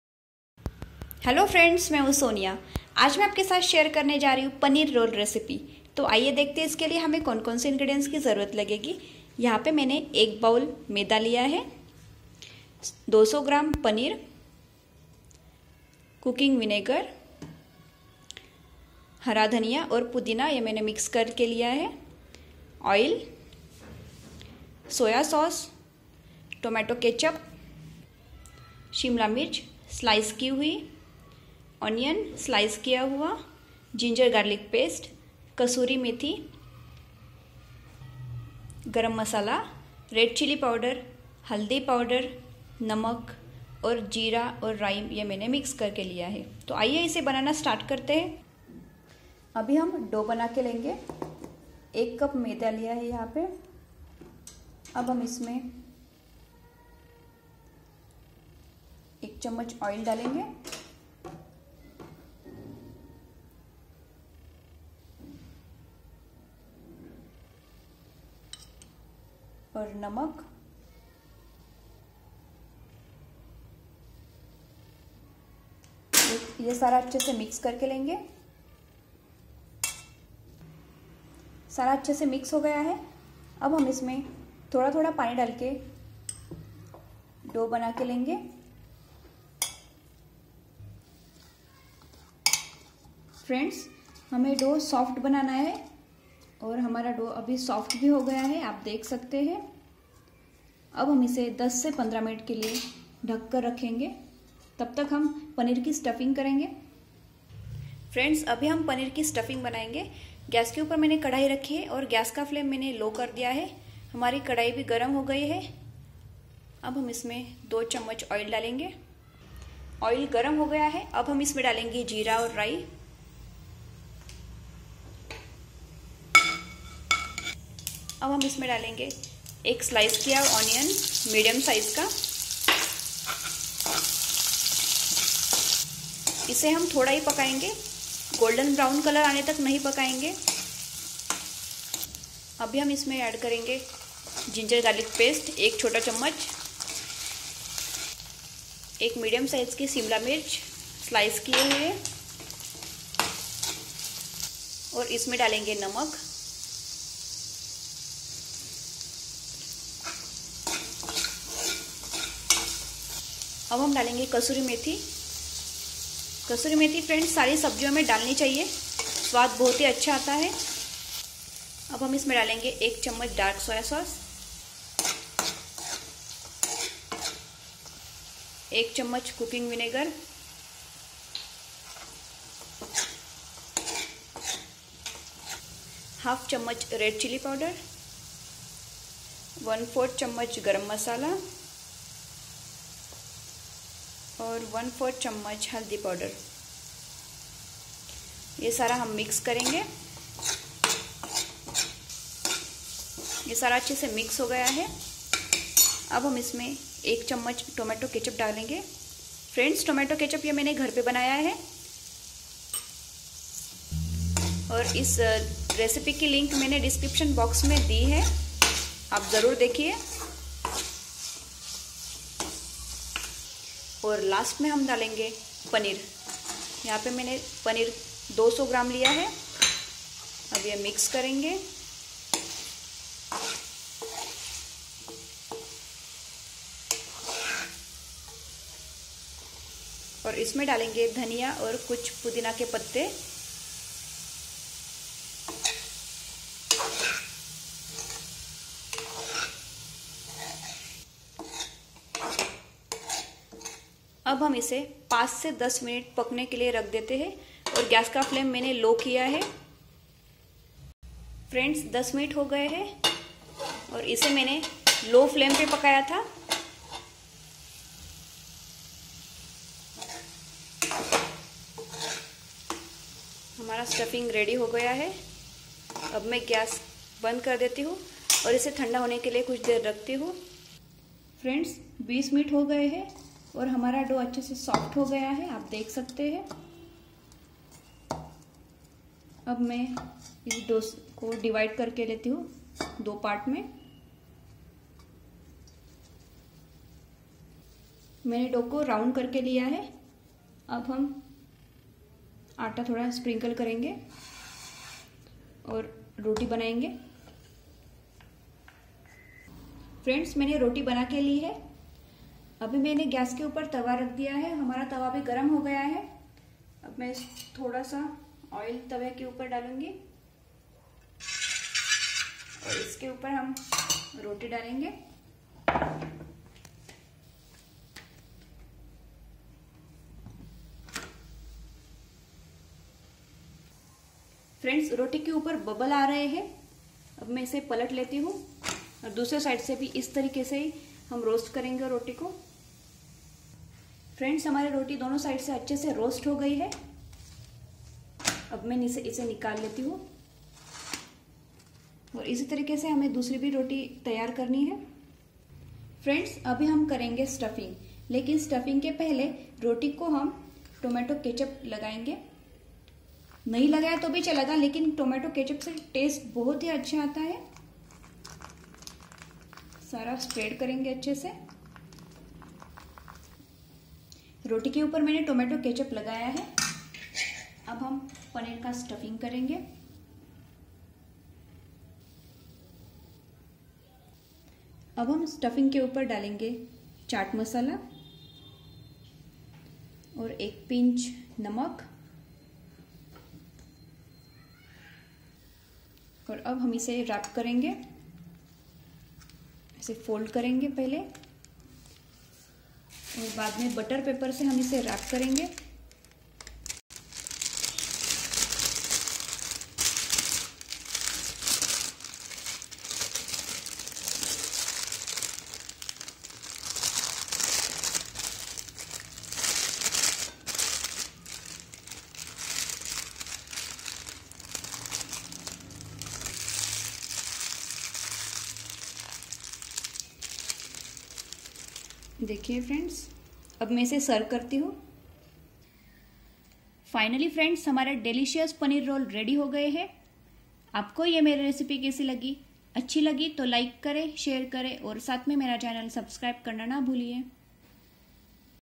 Paneer Roll Recipe in Hindi | Homemade Tasty Paneer Paratha Recipe
Ingredients:
Maida 1 Bowl,
Paneer 200 gm,
Cooking Vinegar,
Coriander & Mints Leaves,
Oil,
Soya Sauce,
Tomato ketchup,
Green Capsicum,
Onion,
Ginger/Garlic Paste,
Kasuri Methi,
Garam Masala,
Red Chill Powder,
Turmeric Powder,
Cumin Seeds, Mustard Seeds.

Find procedure to prepare Paneer Roll in this video.